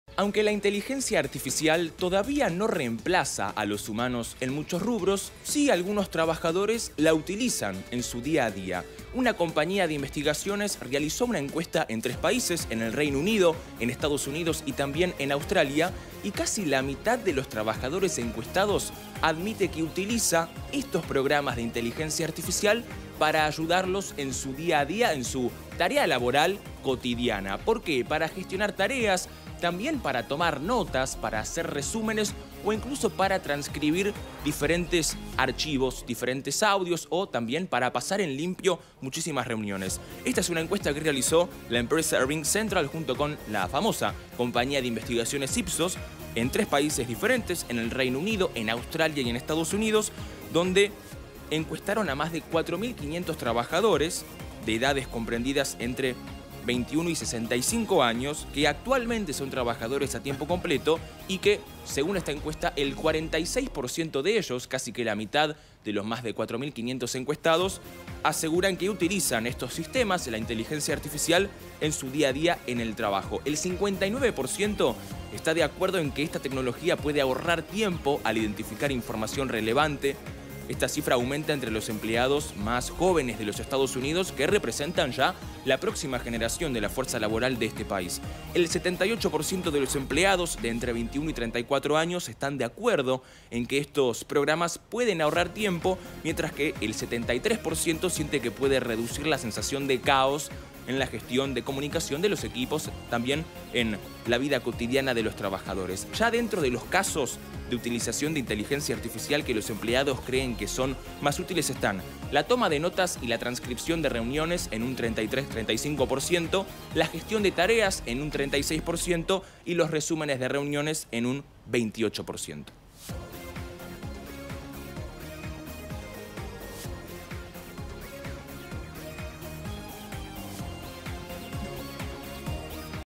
La mitad de los empleados de Estados Unidos, Reino Unido y Australia utilizan IA en sus trabajos
👉 Una reciente encuesta reveló información valiosa sobre el papel determinante que desempeñará la inteligencia artificial en el futuro de la productividad y las comunicaciones en los espacios laborales.